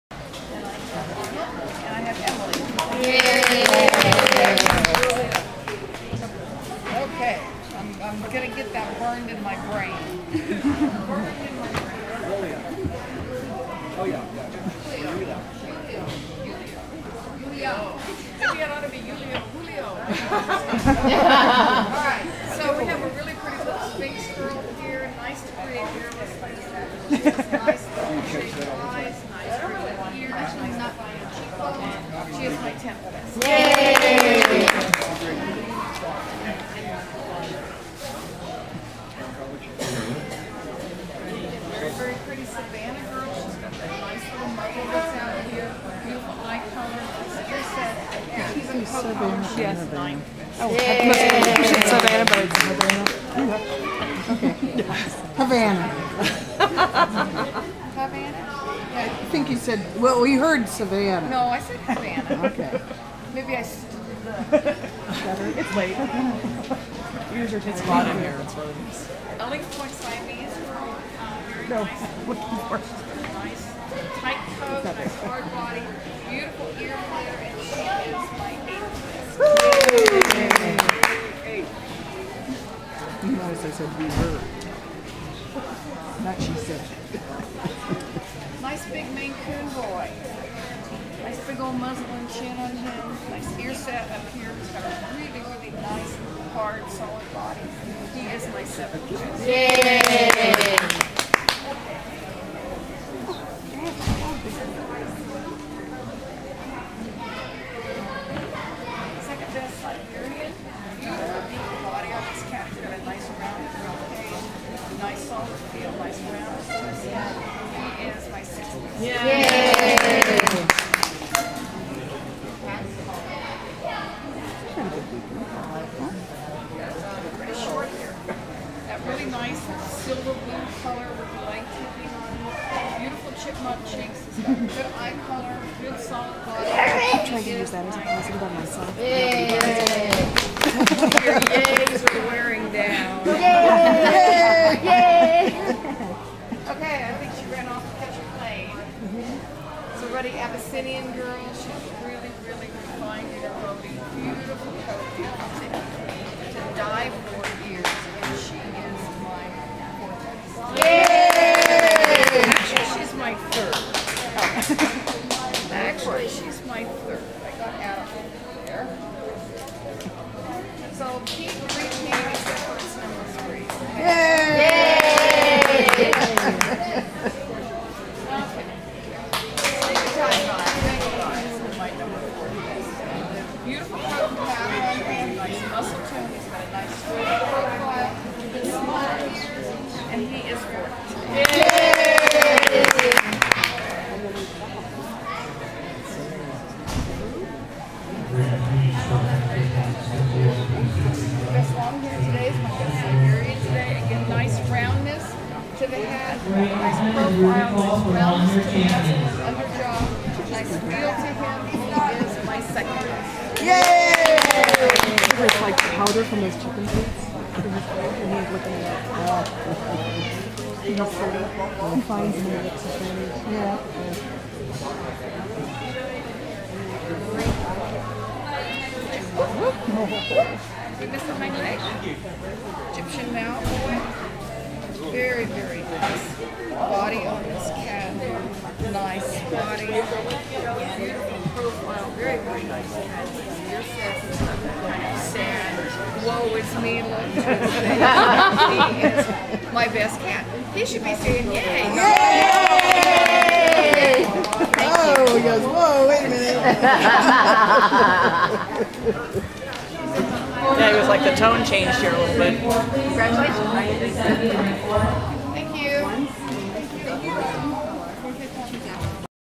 TICA Cat Show AB Final in Salt Lake City, UT, October 09-10, 2010 Judge Bobbie Tullo
Salt Lake City, UT, October 09-10, 2010
All Breed Final of judge Bobbie Tullo; Host Utah's Royal Felines Cat Show. Jyntico Alex of Siberianmeow receiving 6th place.
for more videos and info